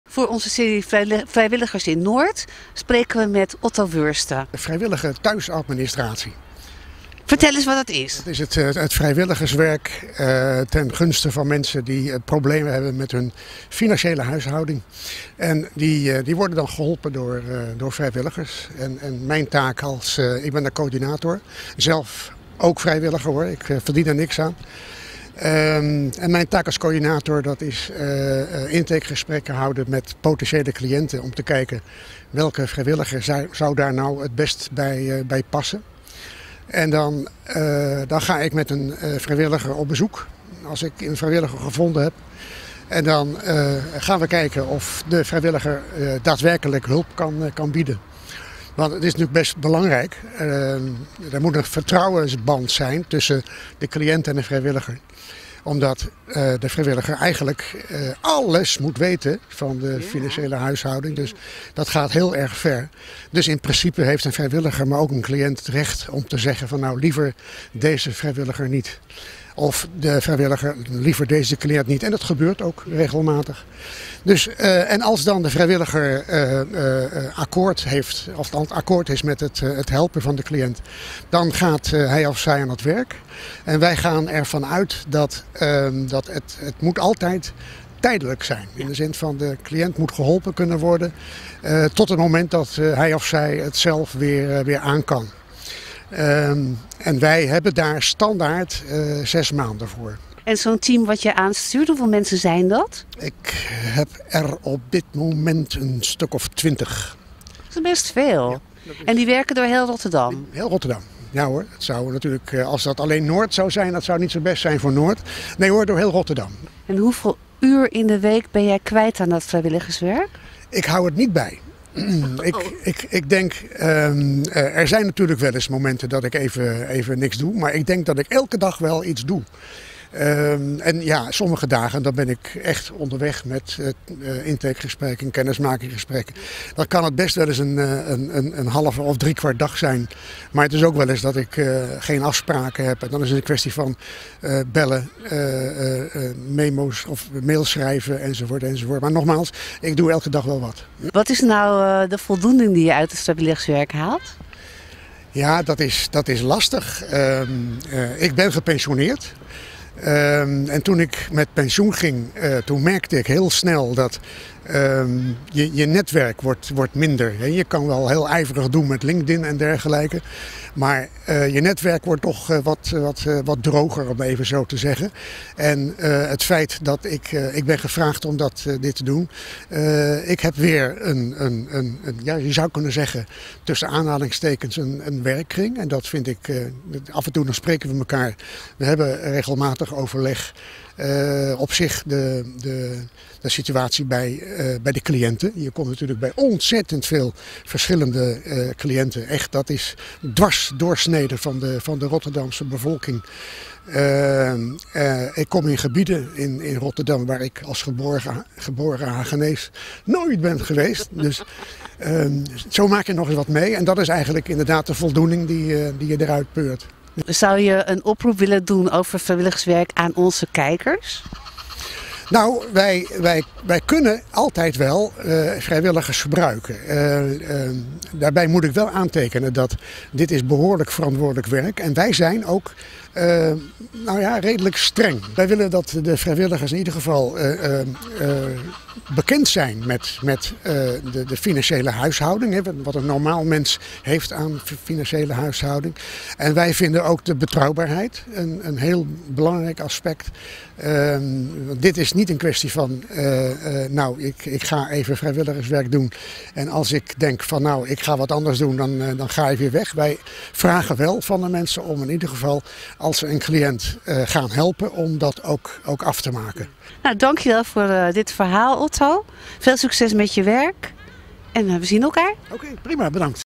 Cineac Noord - Vrijwilligers 2 Otto Wursten
Otto Wursten is de tweede vrijwilliger uit onze nieuwe serie.